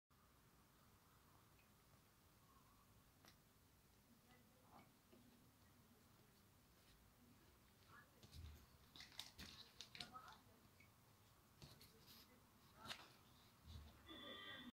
Araç İçi Vantuzlu Yapışma Oto Pusula Büyük Boy Işıksız
BÜYÜK BOY VANTUZLU PUSULA

Kaliteli canlı yeşil renkli pusula. Işıksızdır. Vantuz ile cama yapıştırılır. Vantuzun ek kısmı metal mafsallıdır. 6,5 cm çapında 4,5 cm yüksekliğindedir. Düz halde 10 cm uzunluğundadır.